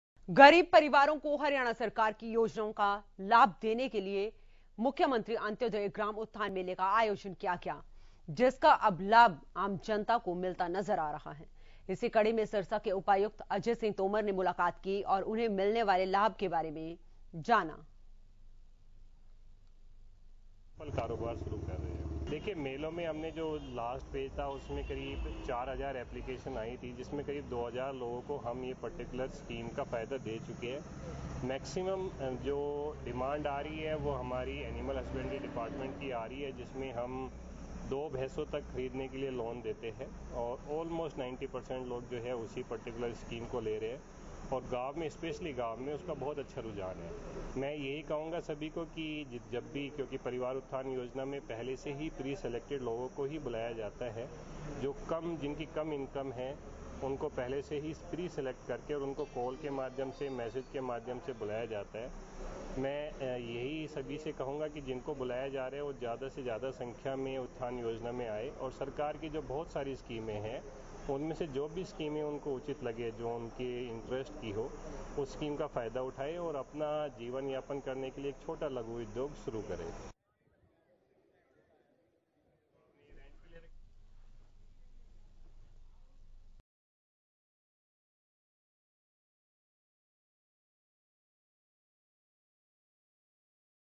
सिरसा: अंत्योदय मेले से लोगों को मिला लाभ, लाभार्थियों से मिलने पहुंचे उपायुक्त
गरीब परिवारों को हरियाणा सरकार की योजनाओं का लाभ देने के लिए के लिए मुख्यमंत्री अंत्योदय ग्राम उत्थान मेले का आयोजन किया गया था.